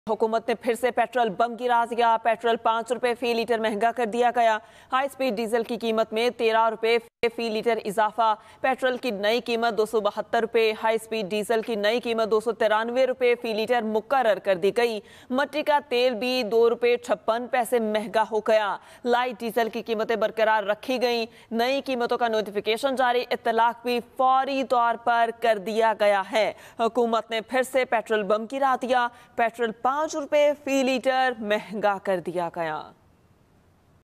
Petrol Price Increased In Pakistan? | Petrol New Price? - IMF | Breaking News | Express News
Petrol Price Increased In Pakistan? | Petrol New Price? | Shehbaz Govt Failed?